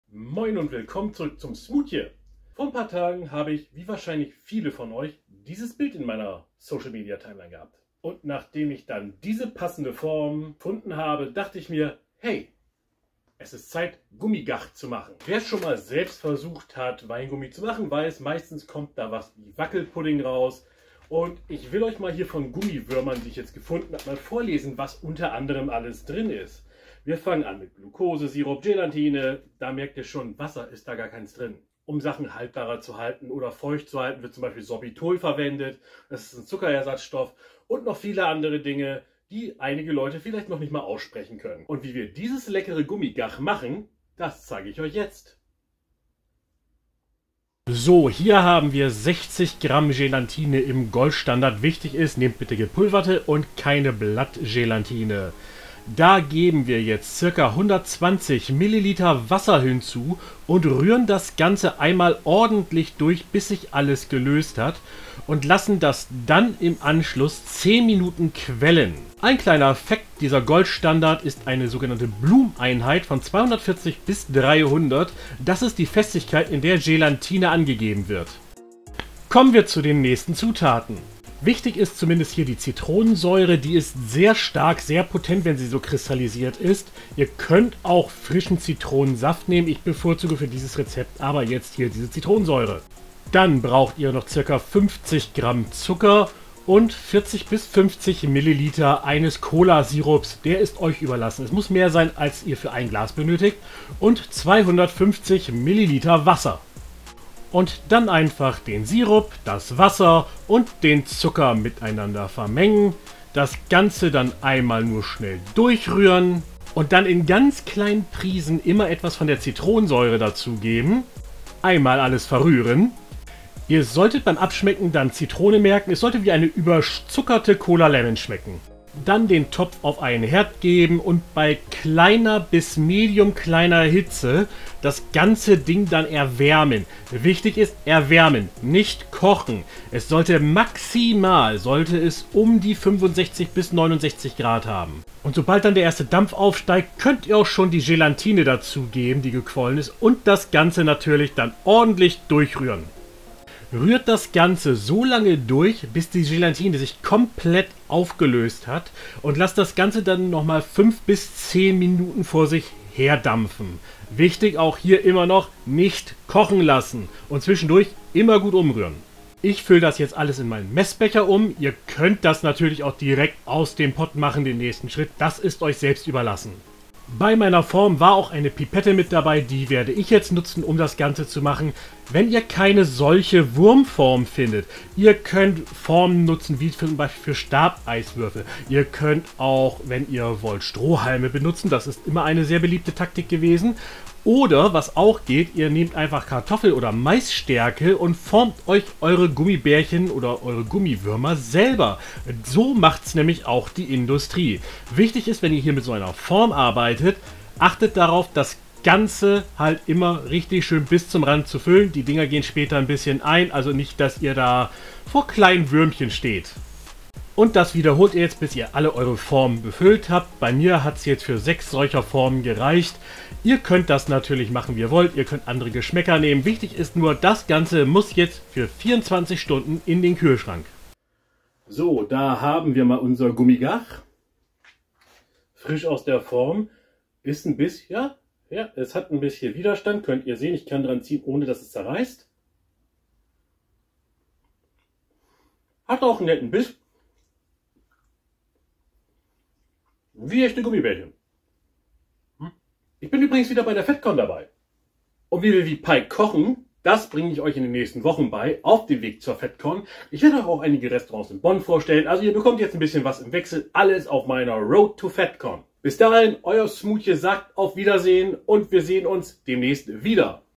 Gummiwürmer einfach selber machen!
Vor einigen Tagen auch das Gummi Gagh gesehen?  Wartet nicht auf Haribo, das sie diese klingonische Köstlichkeit herstellen!  Euer Smutje zeigt euch heute wie ihr das macht.

Ihr braucht:
60 gr Gelantinepulver
370 ml Wasser
50 ml Colasirup
50 gr Zucker
Etwas Zitronensäure

0:00 Intro
0:47 Galantine quellen lassen
1:18 Zutaten
1:47 Zubereitung
2:25 Kombinieren
2:50 Abfüllen
3:50 Tastetest
4:13 Outro